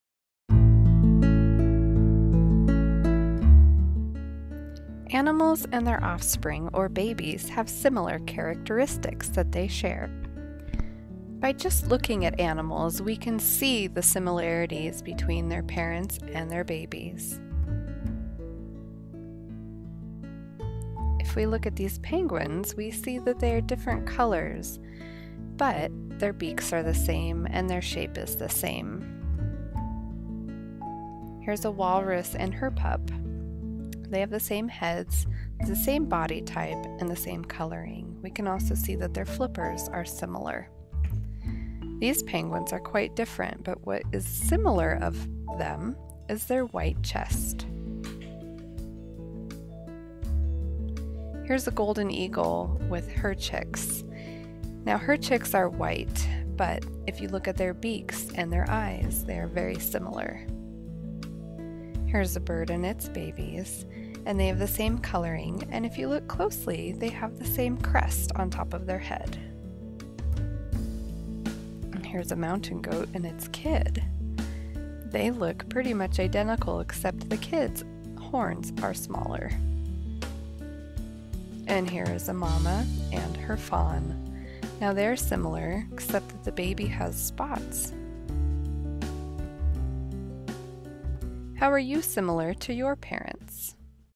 Animals and Offspring
A quick video highlighting similarities between animals and their offspring.